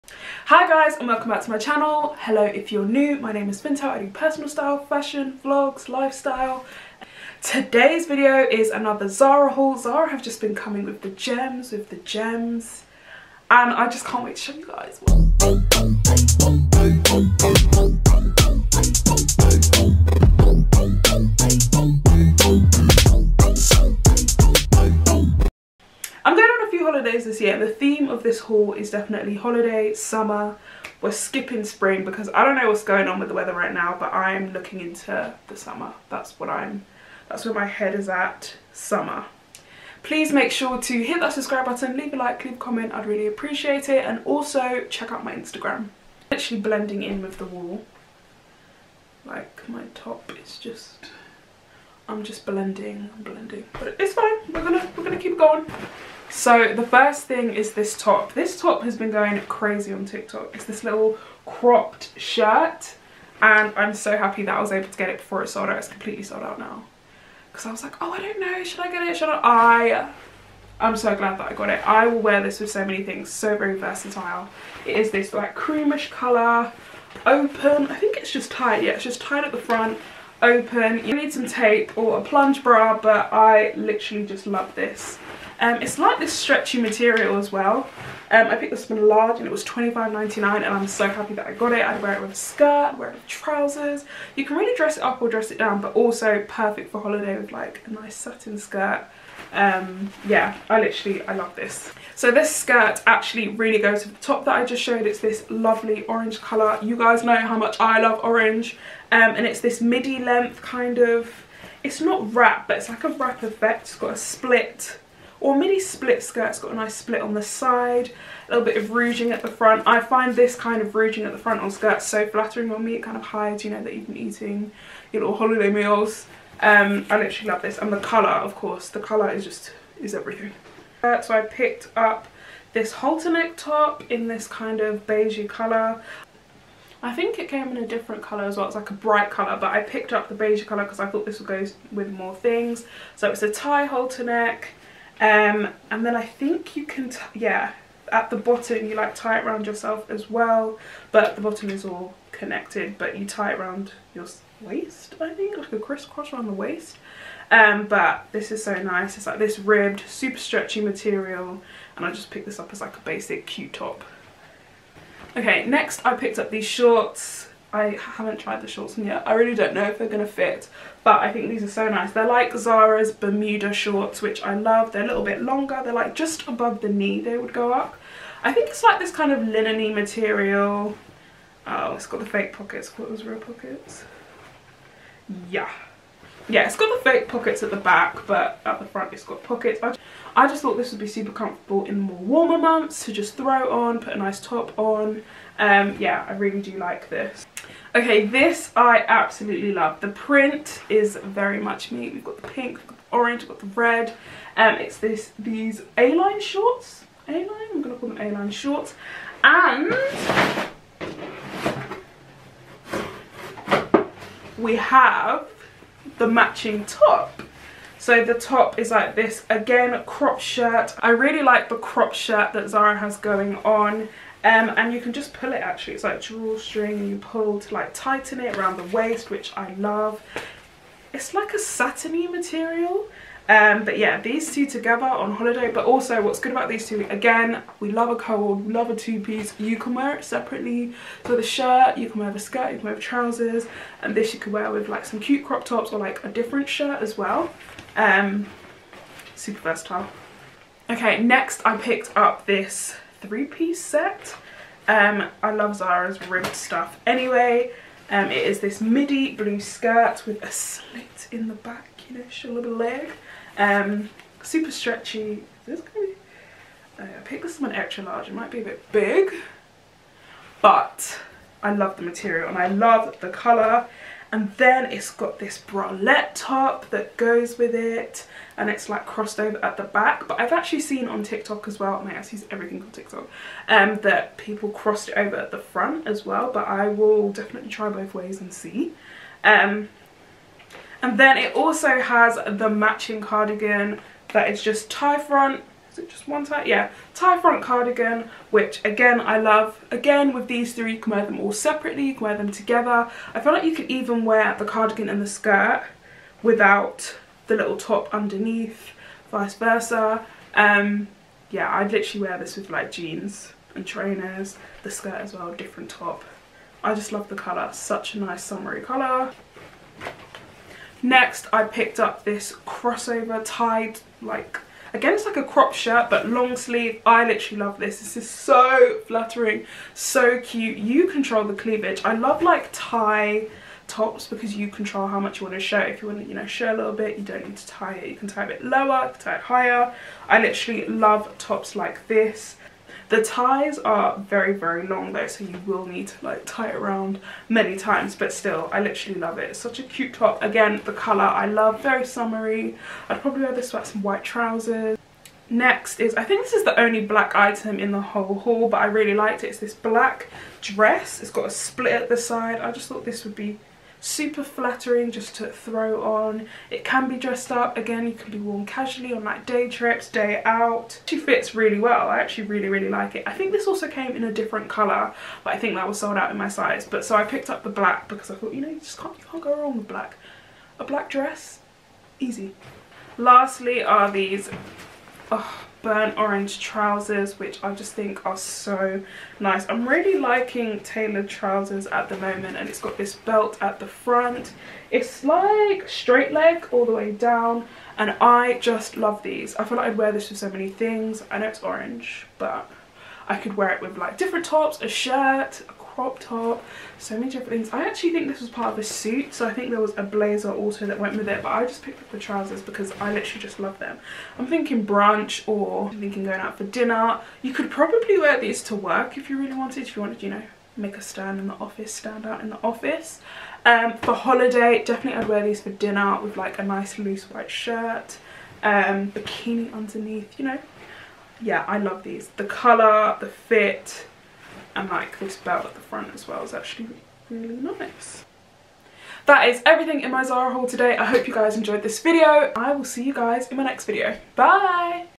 NEW IN SPRING ZARA TRY ON HAUL | Funto's Flair
Hey Everyone! Todays videos is a Zara haul, they current have such cute gems

Funto's Flair TikTok- Funtosflair